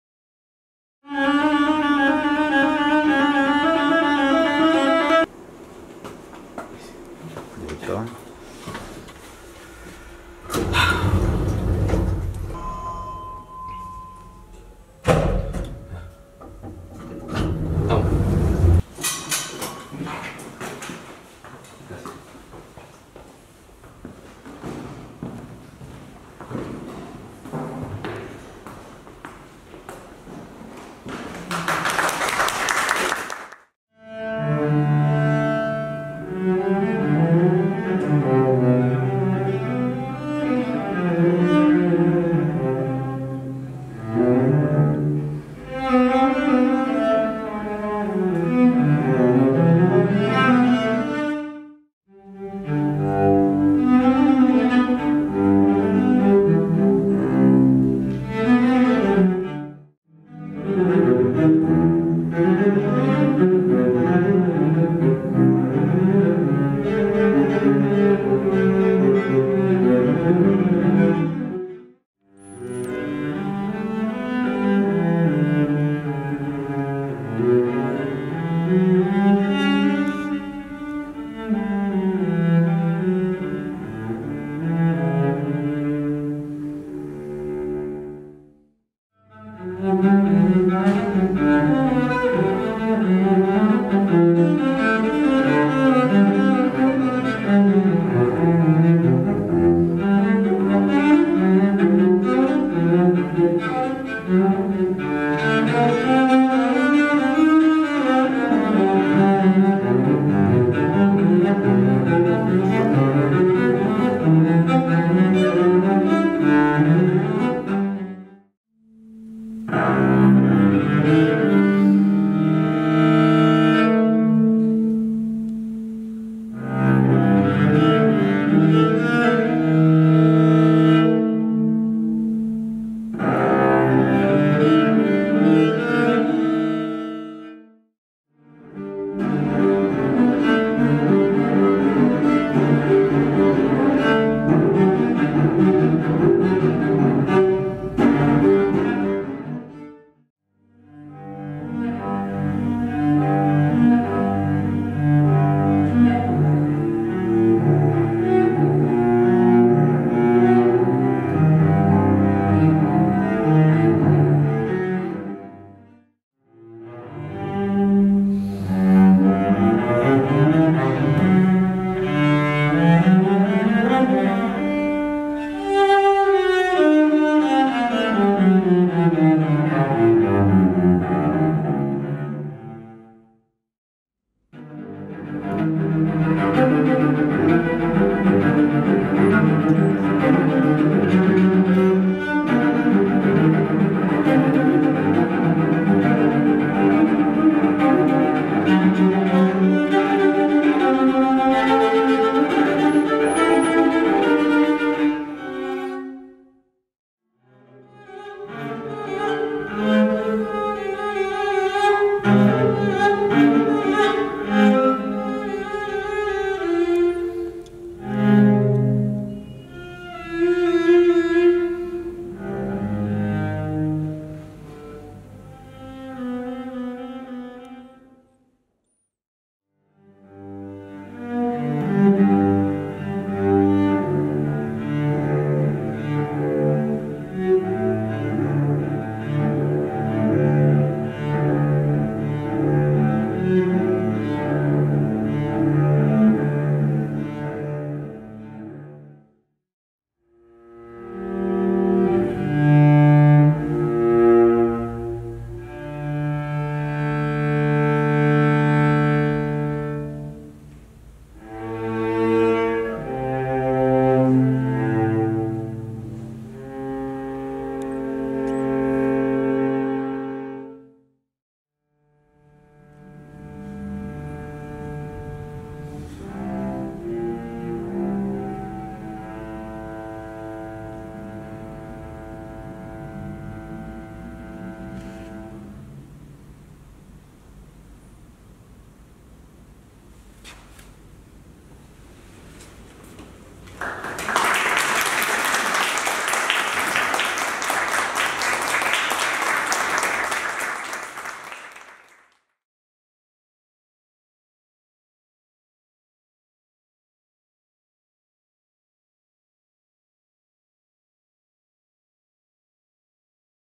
Ramón Poveda Arriati - CELLO ANACRÓNICO - REEL
Resumen del concierto "Cello Anacrónico" realizado el 18/05/2022 en el Museo Benjamín Vicuña Mackena

Violoncello: Ramón Poveda Arriati
Cámara: Oscar Flores